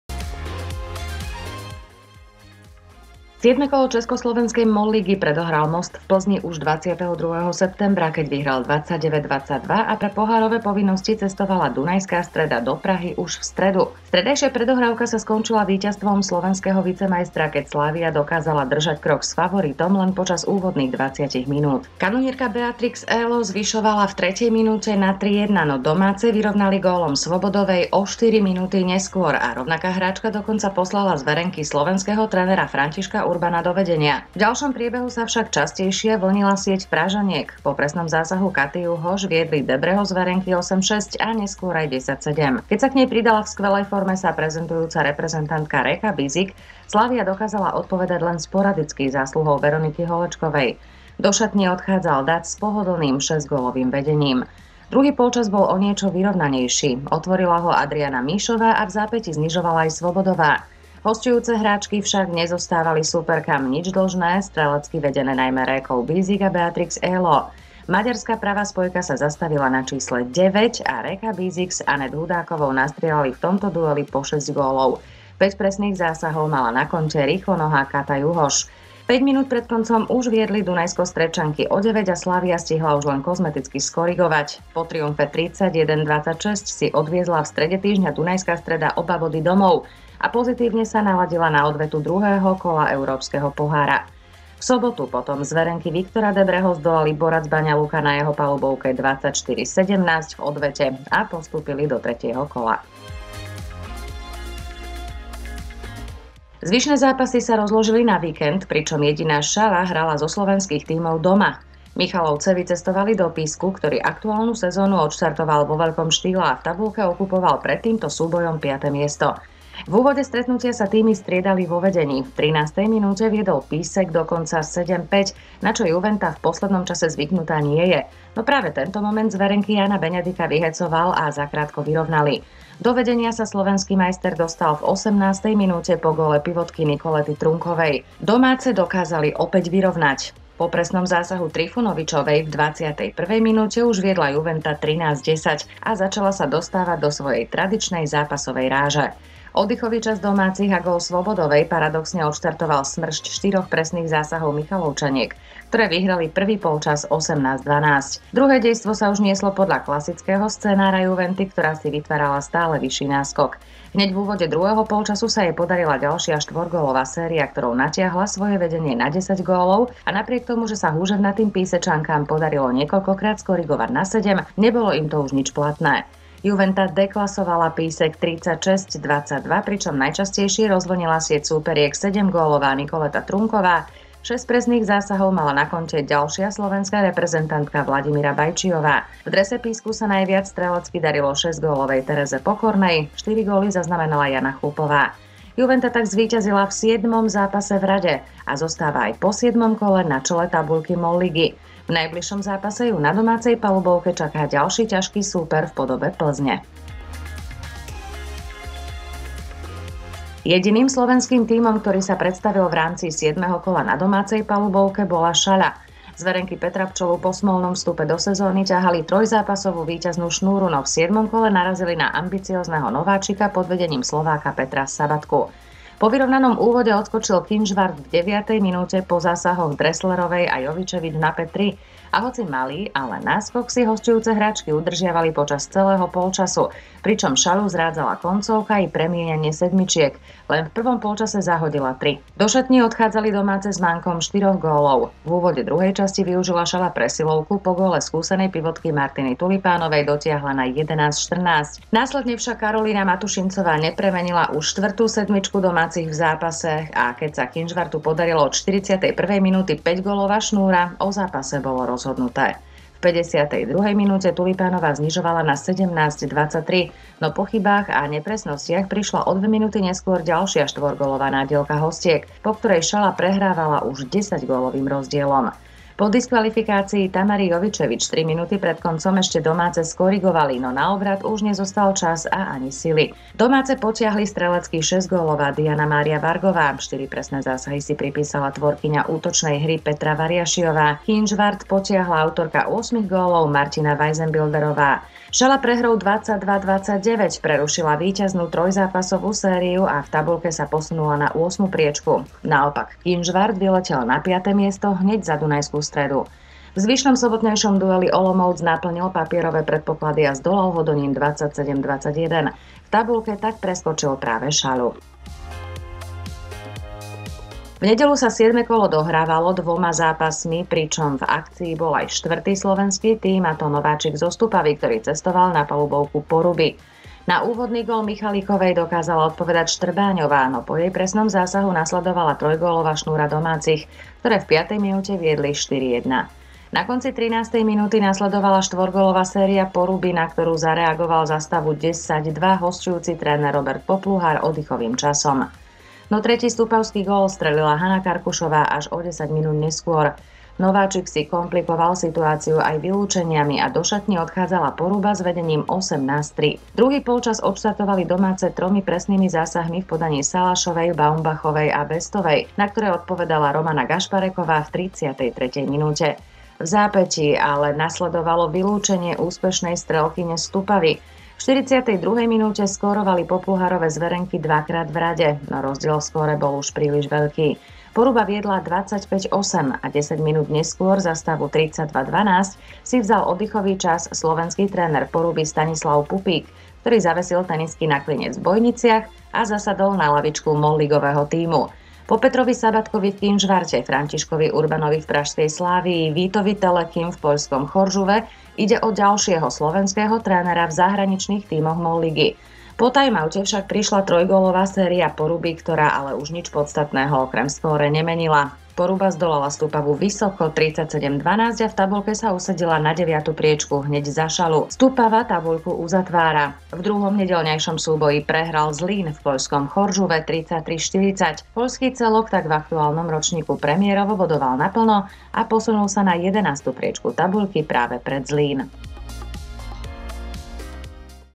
Zostrih z MOL ligy 25.10.2021
Zostrih toho najlepšieho zo 7. kola MOL ligy 2021/2022